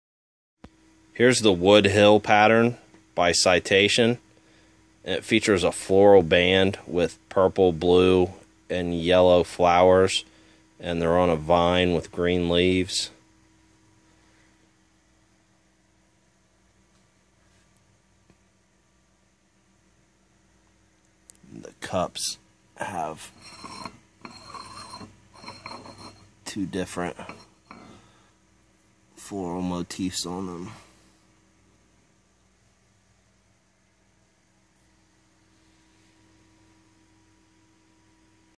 Citation - Woodhill Pattern Stoneware China
Match retired Citation - Woodhill pattern stoneware China here! Features Cream Base With a Multi Colored Floral band.